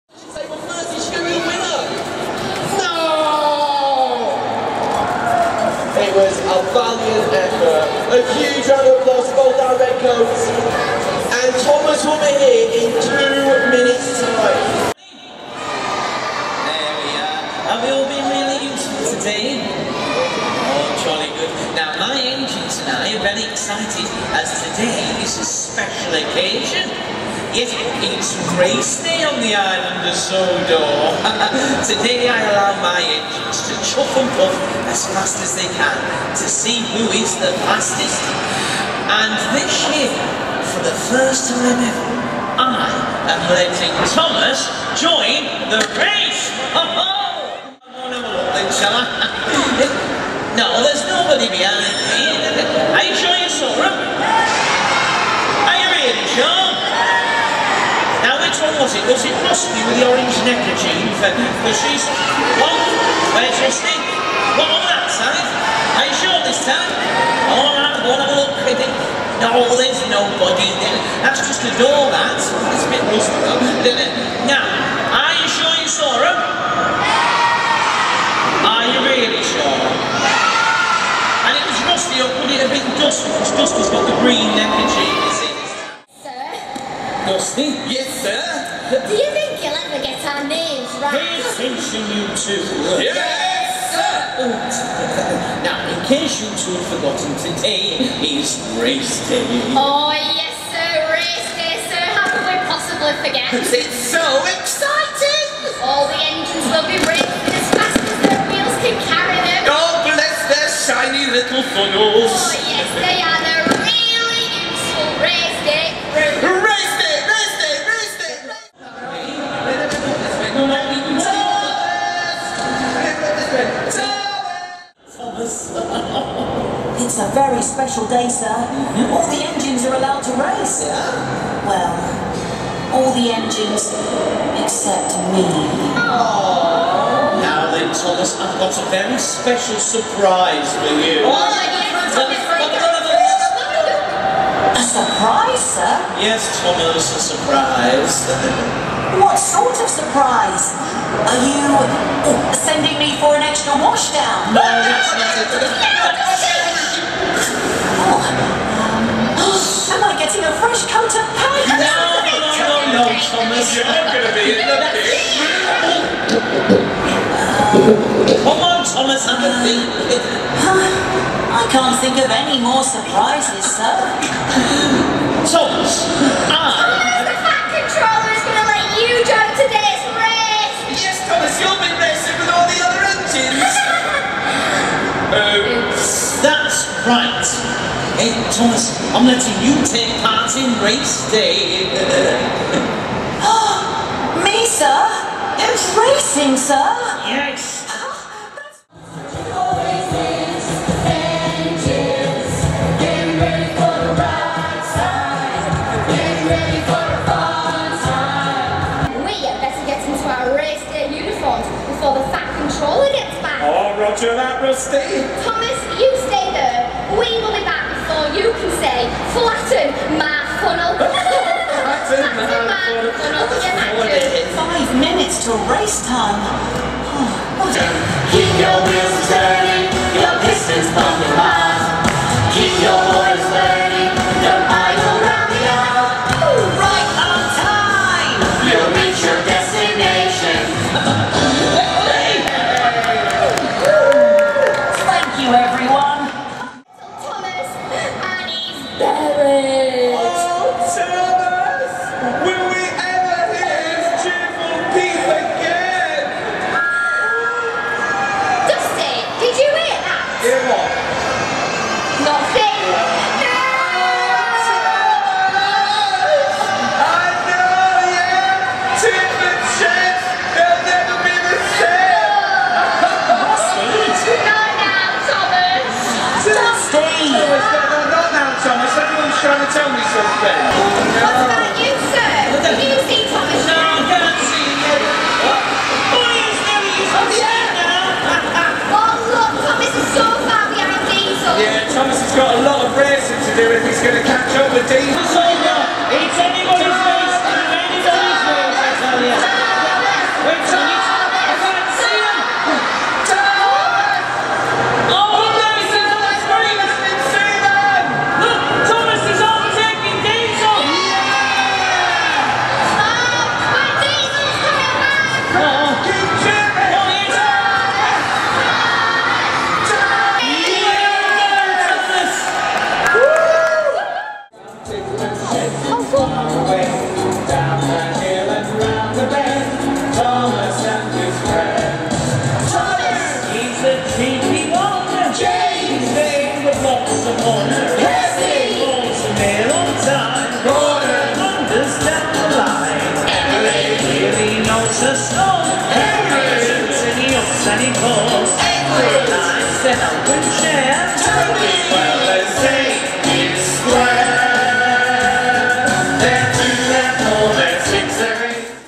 Butlin's Minehead - Thomas the Tank Engine. September 2016.
A few scenes from the Thomas the Tank Engine show at Butlin's, Minehead. September 2016.